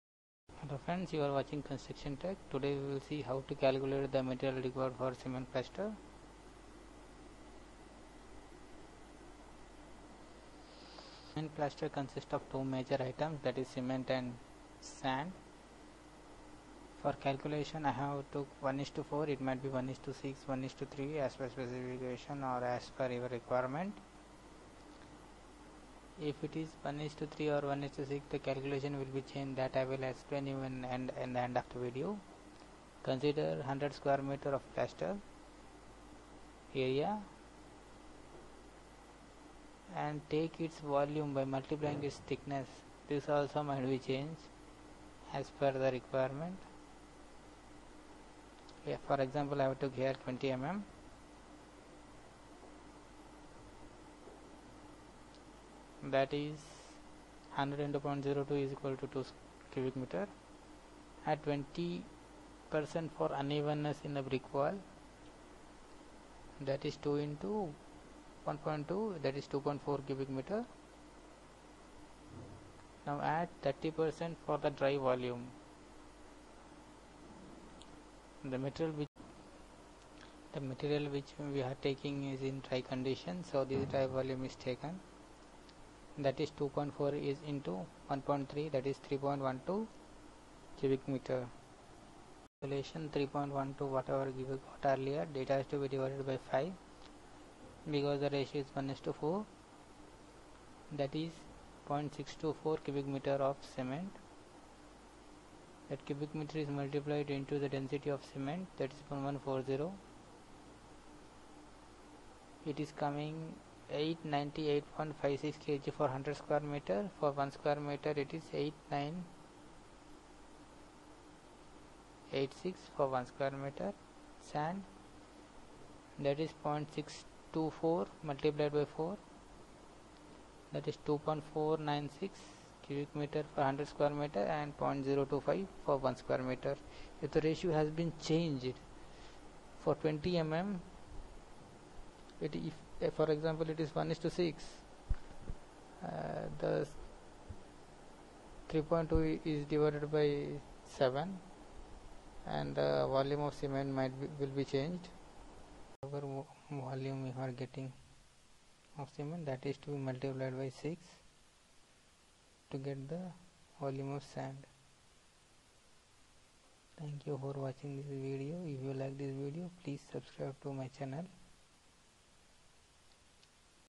Cement required for plastering per sq m
cement plastering calculation
Requirement for Sand and cement is calculated in this video this will help to understand how much quantity of cement  and sand required for 100 Sqmtr/1 sqmtr